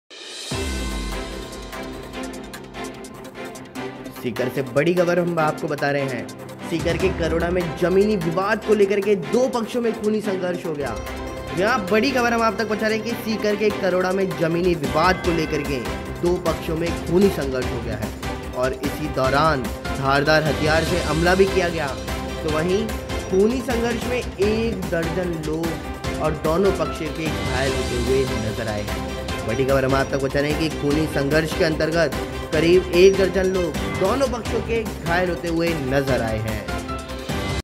जमीनी विवाद को लेकर दो पक्षों में खूनी संघर्ष Bloody conflict between two sides over ground dispute
सीकर के करोडा में जमीनी विवाद को लेकर दो पक्षों में खूनी संघर्ष हो गया, इस दौरान धारदार हथियार से हमला किया गया, खूनी संघर्ष में करीब एक दर्जन लोग दोनों पक्षों के घायल हुए हैं।

In Sikar's Karaoda, there was a bloody struggle between the two sides over the ground dispute, in the meantime, attacked with sharp weapons, about a dozen people were injured in the bloody conflict.